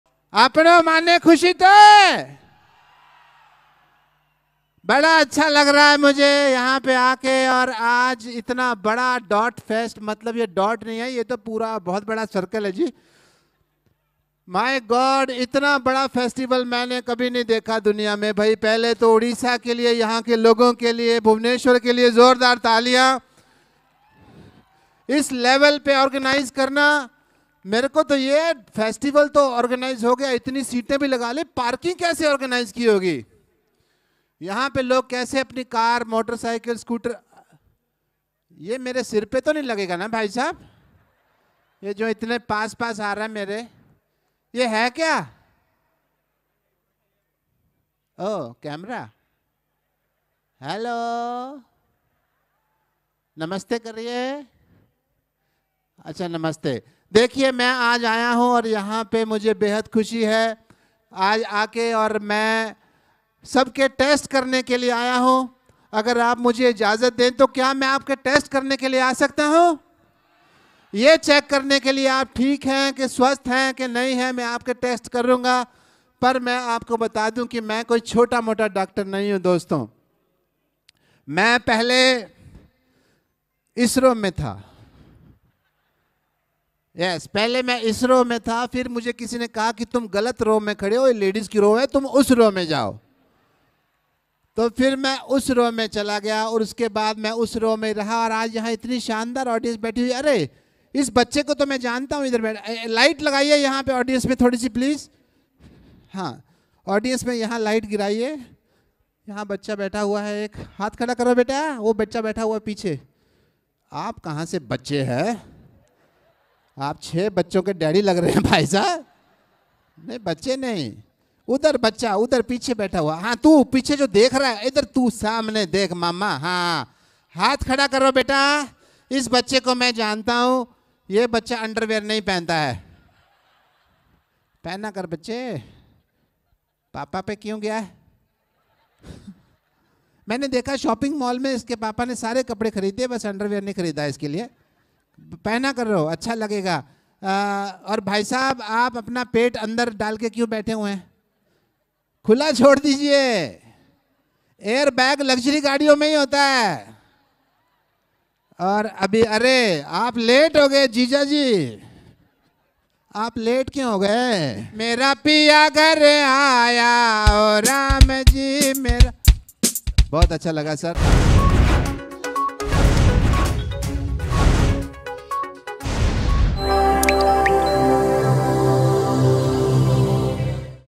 Dot FEST Bhubaneswar | ଆପଣମାନେ ଖୁସି ତ....କହିଲେ ସୁନୀଲ ଗ୍ରୋଭର | Sunil Grover
ଭୁବନେଶ୍ଵର ଡଟ୍‌ଫେଷ୍ଟ୍‌ରେ ଆପଣମାନେ ଖୁସି ତ. କହିଲେ ସୁନୀଲ ଗ୍ରୋଭର. ।